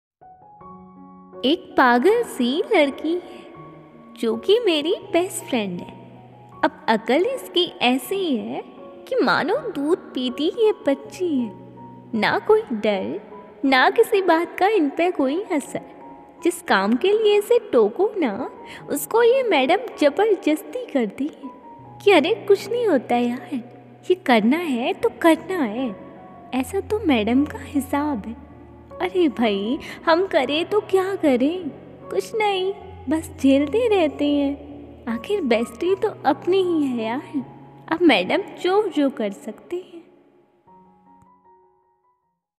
Ek Pagale Si Best Friend🤦 | Best Friend Poetry | Bestie Whatsapp Status | Nikita's Poetry Collection
ek pagale si ladki hai jo ki meri best friend hai ab akal iski esi hai mano dudh piti ye bachhi hai na koi dar na kisi bat ka in pe koi asar jis kam ke liye ise toko na usko ye madem jabardasti karti hai ki are kuchh nahi hota yar ye karna hai to karna hai esa to madem ka hisab hai are bhayi hum karen to kya karen kuchh nahi bas jhelte rehte hai aakhir bestie apni hi hai yar ab madem jo jo kar sakte hain.☹️🙏🏻

this poetry video dedicate to your best friend or bestie ! 😊👍

Video keywords...

Nikita's Poetry Collection
Nikita'sPoetryCollection

I hope you like this video friends 😊